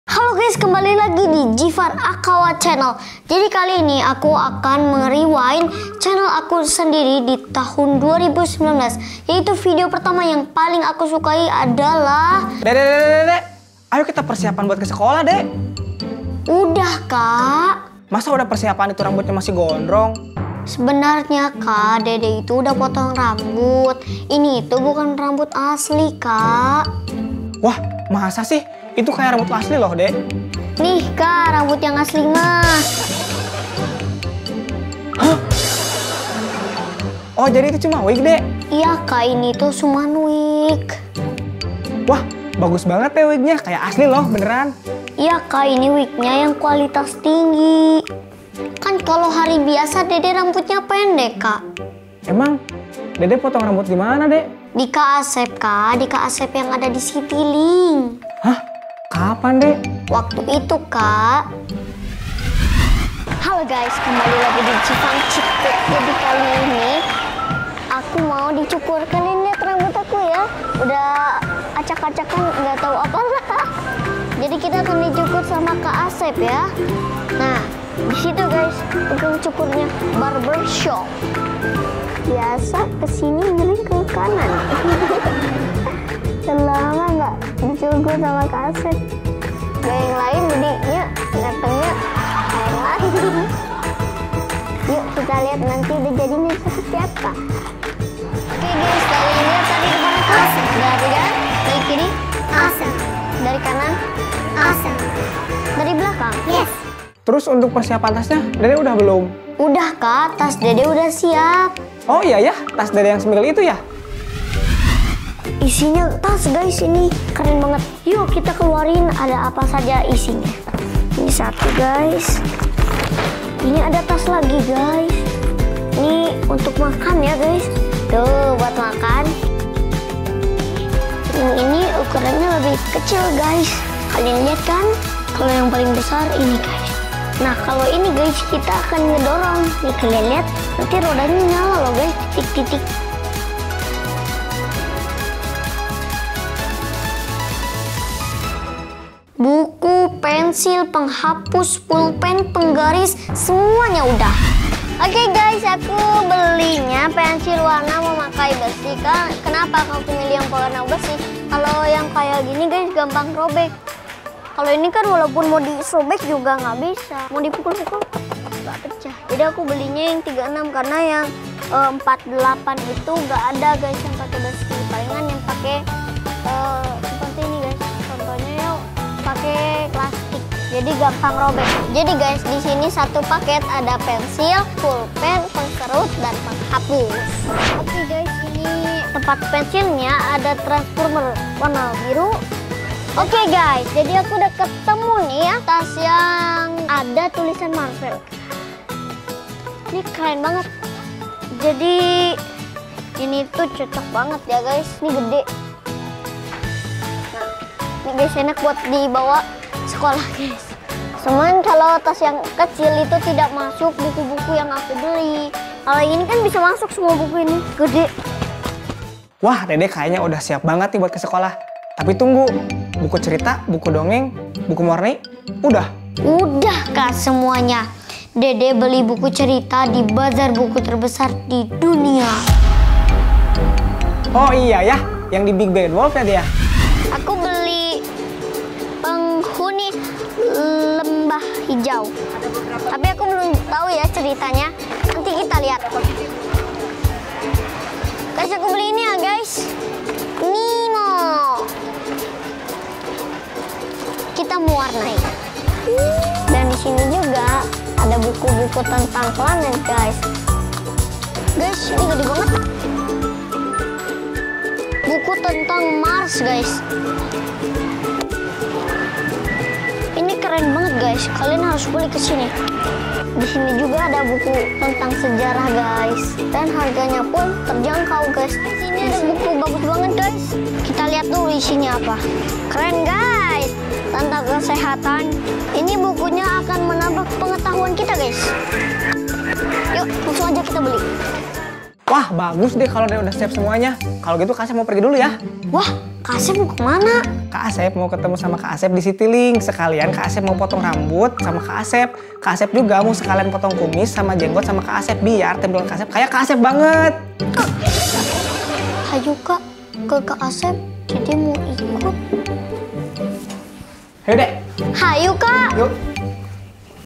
DRAMA | Rewind Jeevan Channel 2019, Back to School, Persiapannya apa saja ?
gess selamat tahun baru 2020,... sekarang aku mau rewind beberapa video yang dah aku buat di tahun 2019 yang bertema back to school 🚸🚸